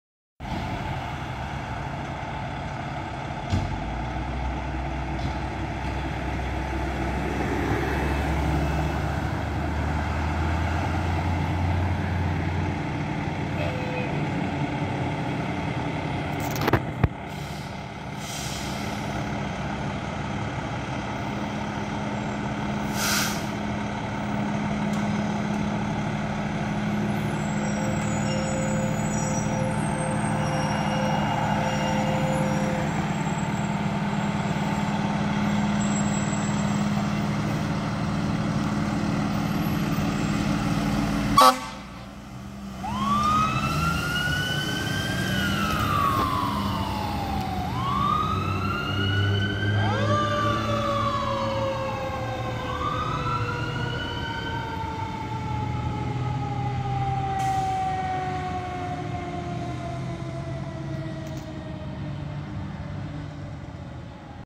Oshawa Fire New Aerial 23 & Pumper 23 Responding
Oshawa fire service Aerial 23 and Pumper 23 responding to an unknown emergency

Apparatus in video

(Aerial 23)

2021 E-One Cyclone HP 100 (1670/250/8F/100' rear-mount platform) (SO# 143525)

(Pumper 23)

2007 Spartan Gladiator Classic LFD / Smeal (1250/500/25A) (SN# 709011) (Ex-Pumper 21)